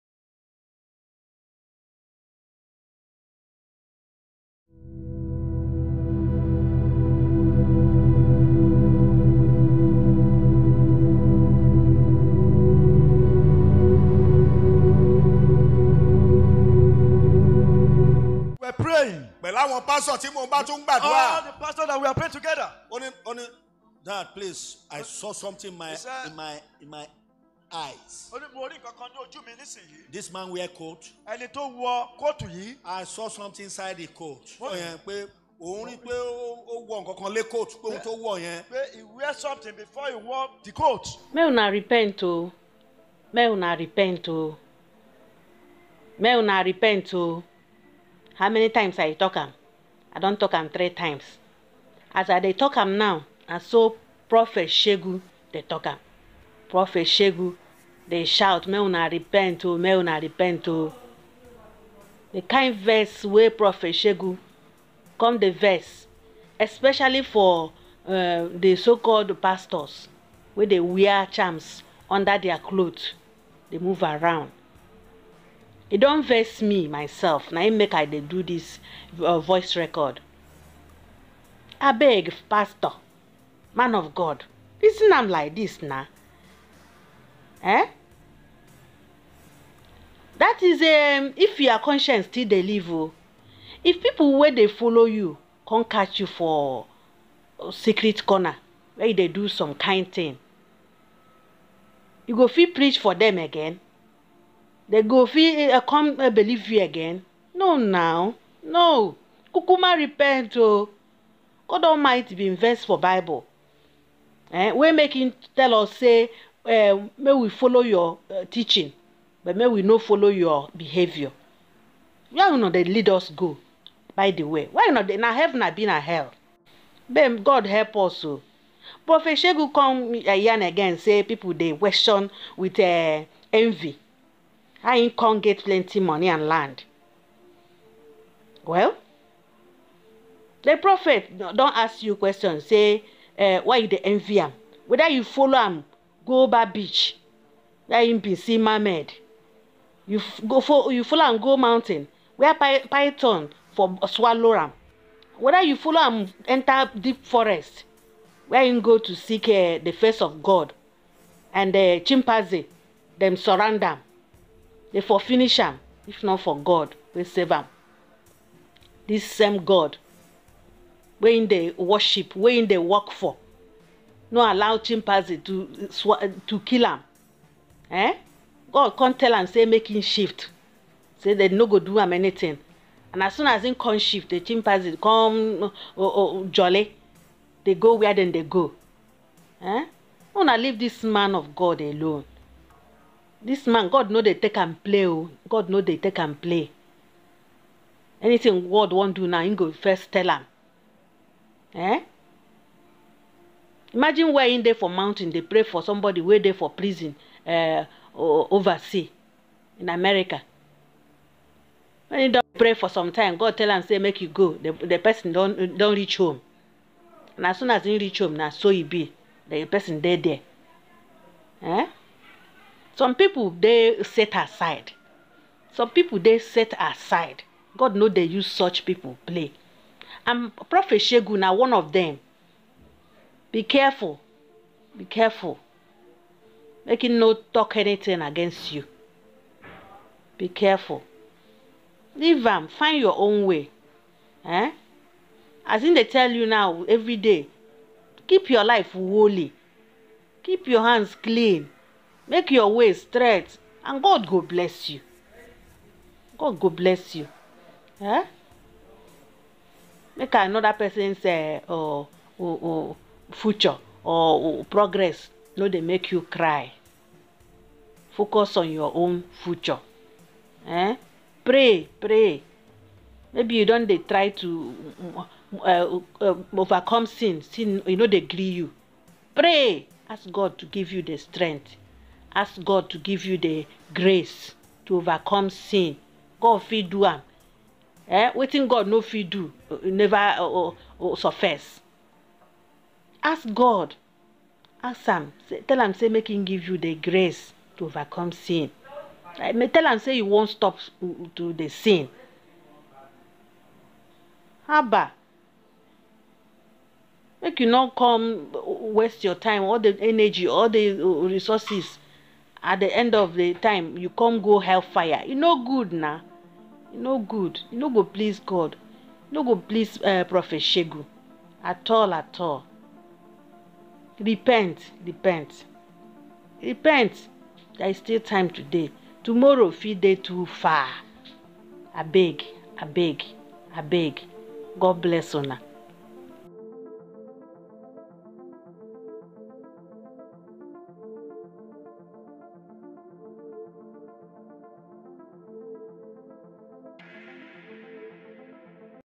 Pastor Caught With Charms/Juju in House of God
A Nigerian pastor was caught with charms/ juju  in the church.